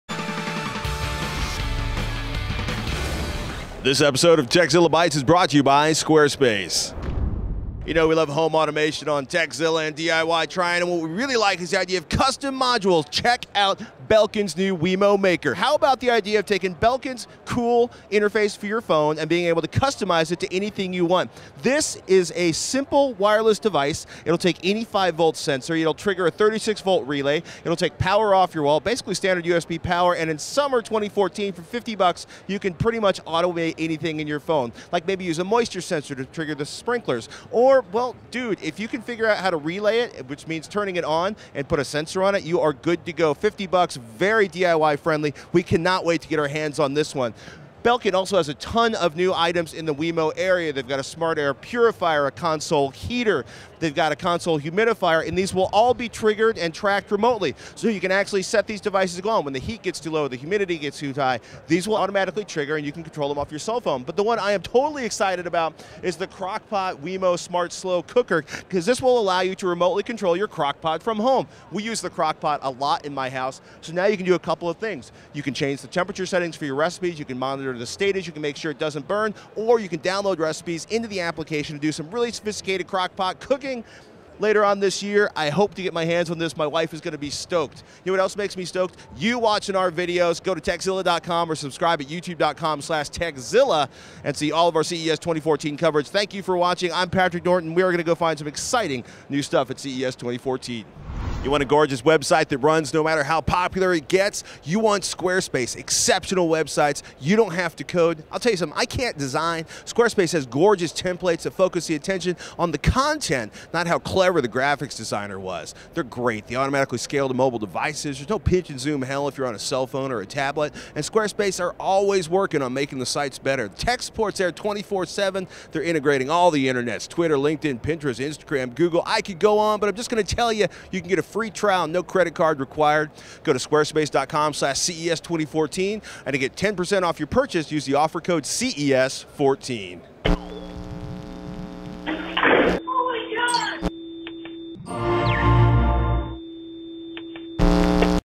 Control Power To Any Device In Your House Wirelessly With the WeMo Maker! - CES 2014
Patrick checks out a few of neat WeMo products from the CES show floor - including a device that lets you control power to anything wirelessly, plus a 'smart' slow cooker for your kitchen!

Watch our CES 2014 coverage in one spot!